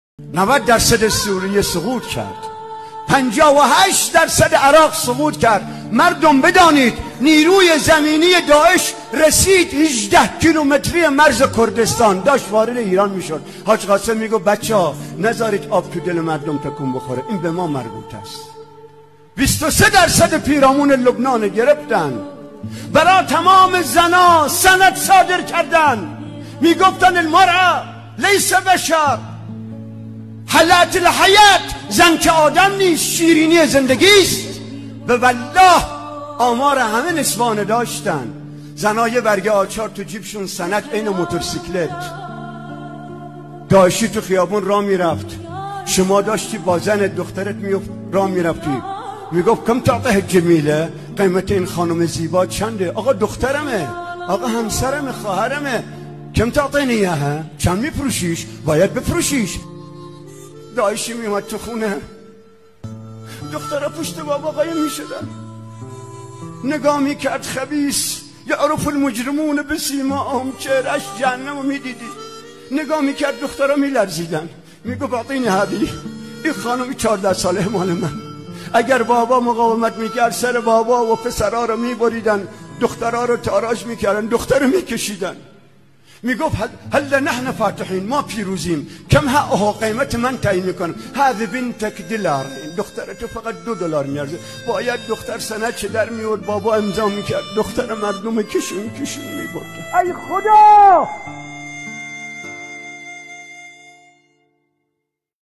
گوشه‌ای از جنایات داعش در سوریه
حاج قاسم سلیمانی چطور برای دفاع از امنیت و ناموس ایران به مقاله با داعش پرداخت؟